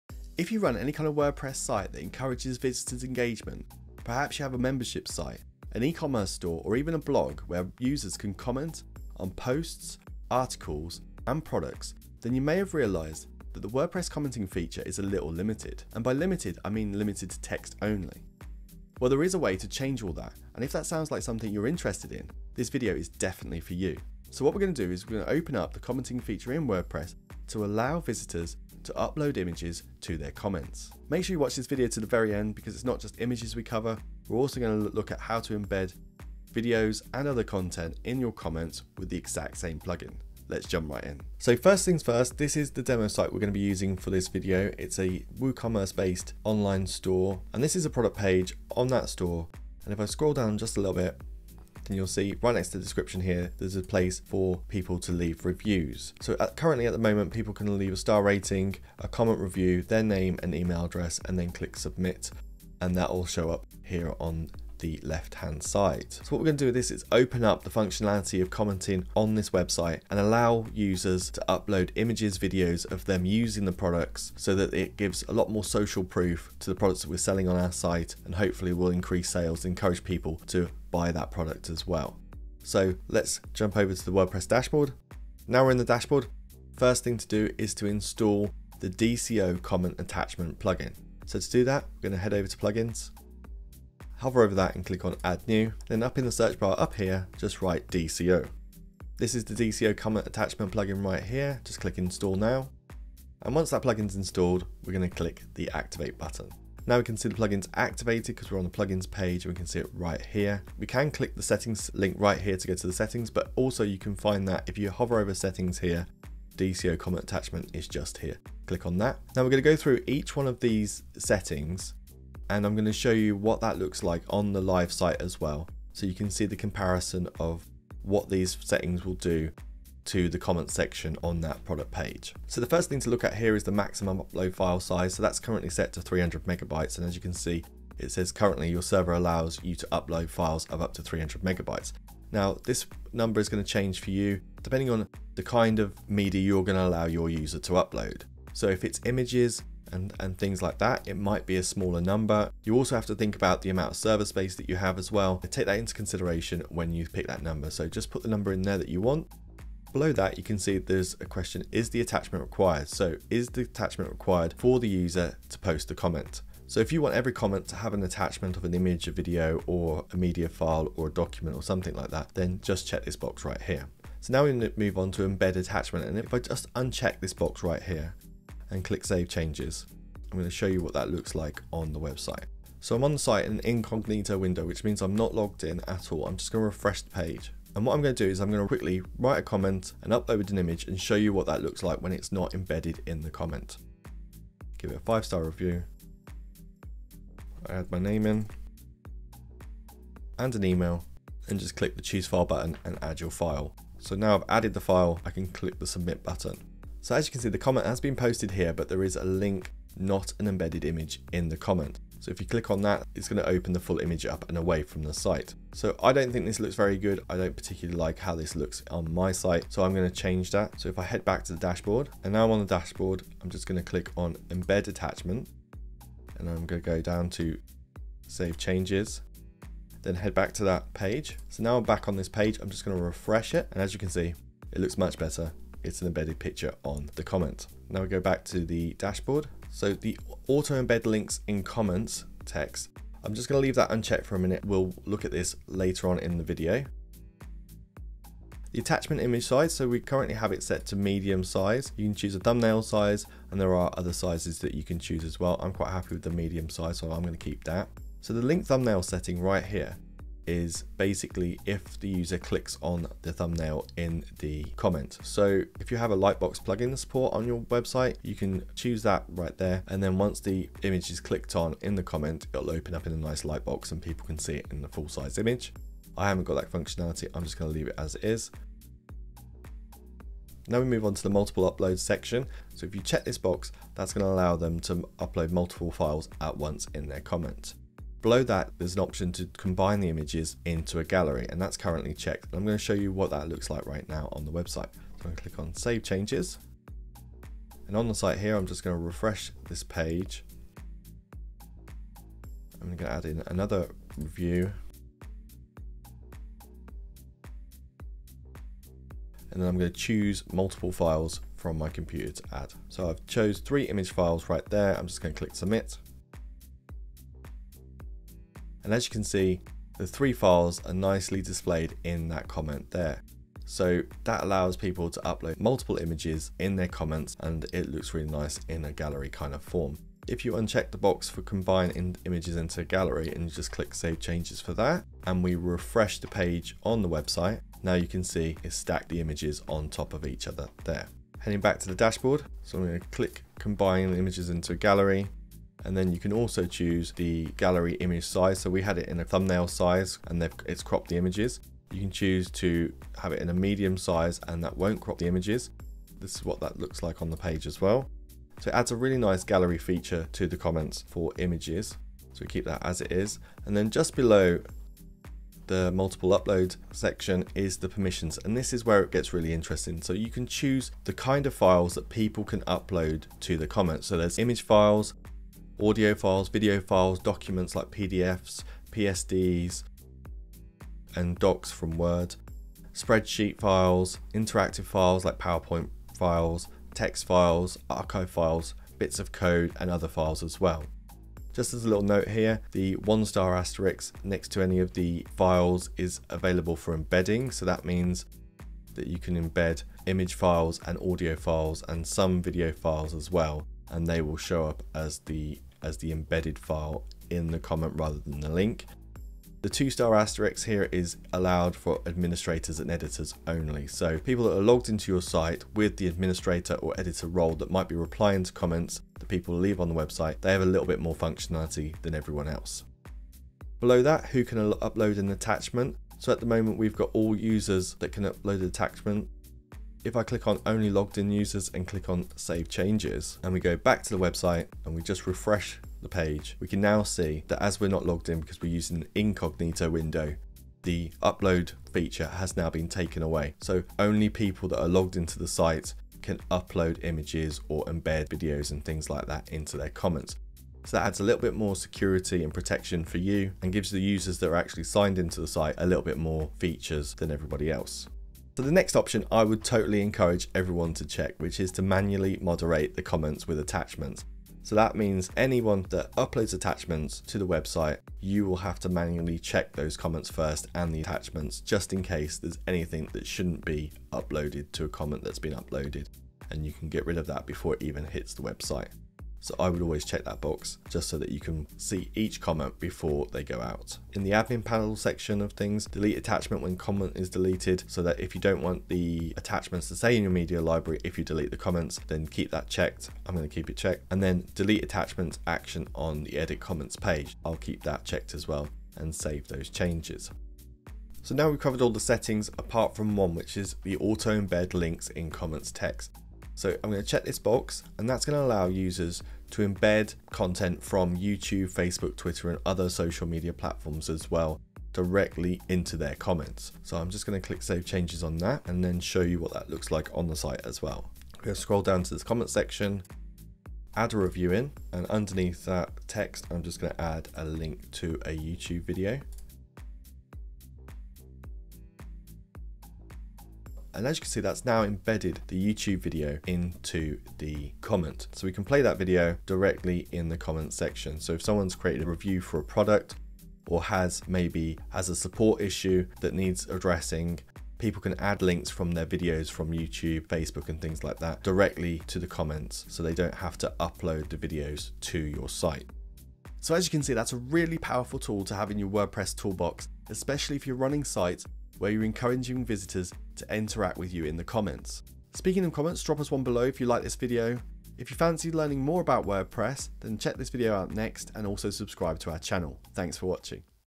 How To Add Image Uploading To Comments on WordPress
Allowing users to upload images and media to their comments on your WordPress site really opens up the possibilities for user engagement.

In this video, we take a look at how to add this feature to your site along with running through different options and configurations to get the functionality you really want.

--Chapters--

00:00 Introduction
01:25 How to Install the DCO Comment Attachment Plugin
01:59 Overview of the DCO comment Attachment Plugin
03:43 Allow Users to Embed Images in WordPress Comments
08:21 Allow Users to Embed Videos and Media in WordPress Comments

--Top Resources--